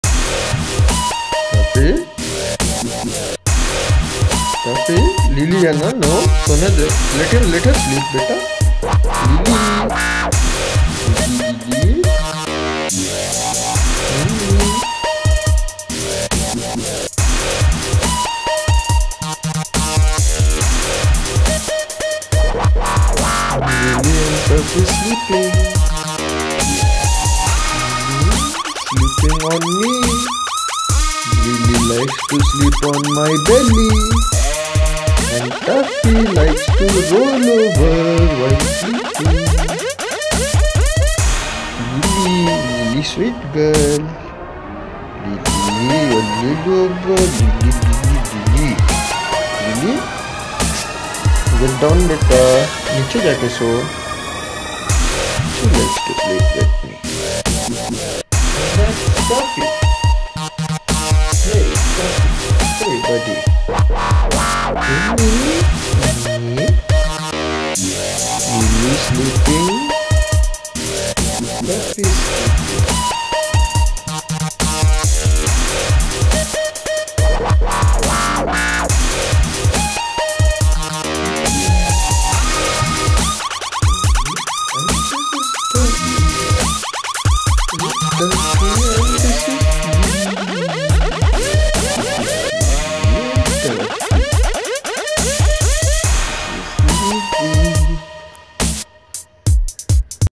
DOG SHARING BED WITH OWNER || CUTE DOGS SLEEPING || FUNNY DOG VIDEOS || TUFFY AND LILY- MUSCULAR DOG
ABOUT THE VIDEO- TUFFFY AND LILY LOVES TO CUDDLE WITH ME ON OUR BED. THEY BOTH ALWAYS SLEEPS BY MY SIDE.

ENJOYING THEIR COMPANY.